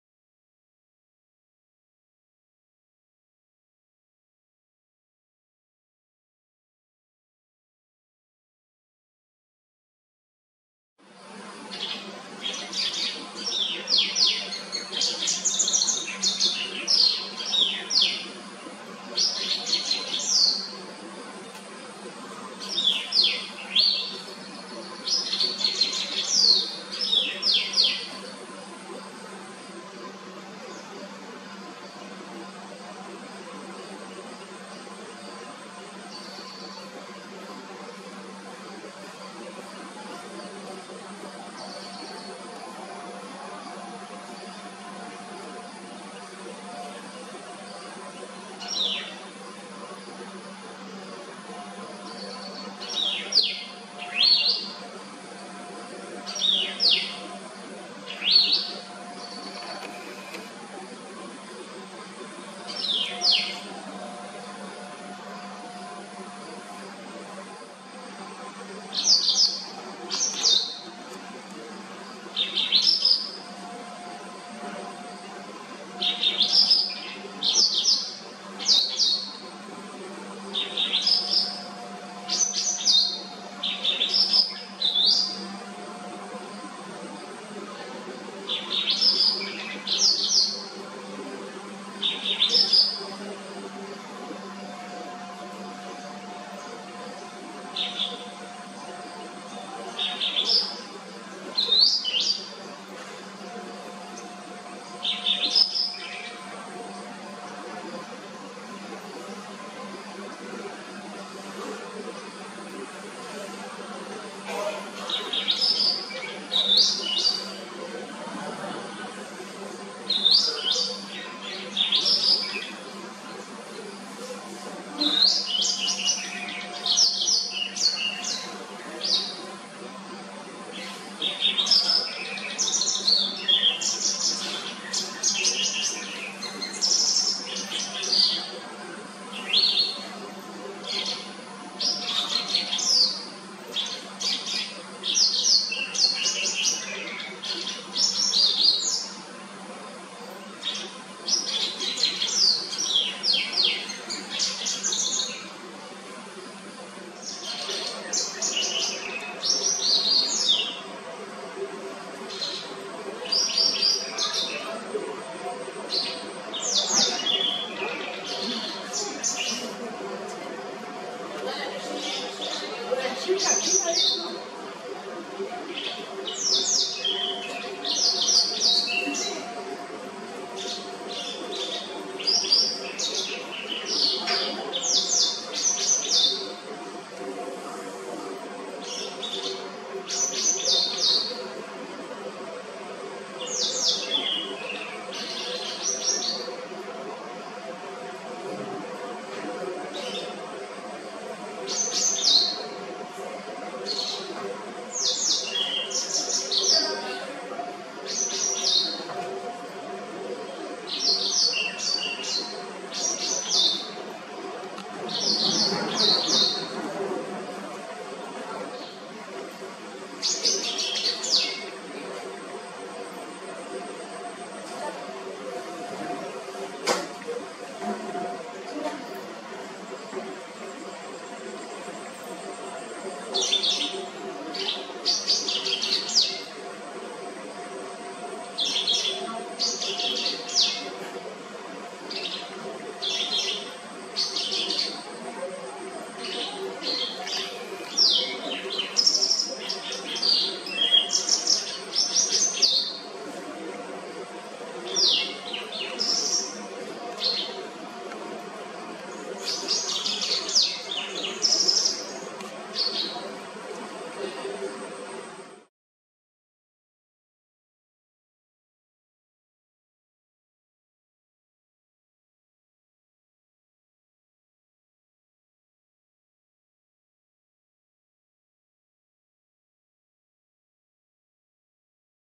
The white browed wagtail | . Its clear, rhythmic chirps and trills create a uplifting atmosphere,
A beautiful bird is captured in mid-song, its beak open wide as it sings a delightful, intricate melody. Each note echoes gently, creating a tranquil moment that celebrates the simple joy of nature’s music. Its clear, rhythmic chirps and trills create a serene, uplifting atmosphere,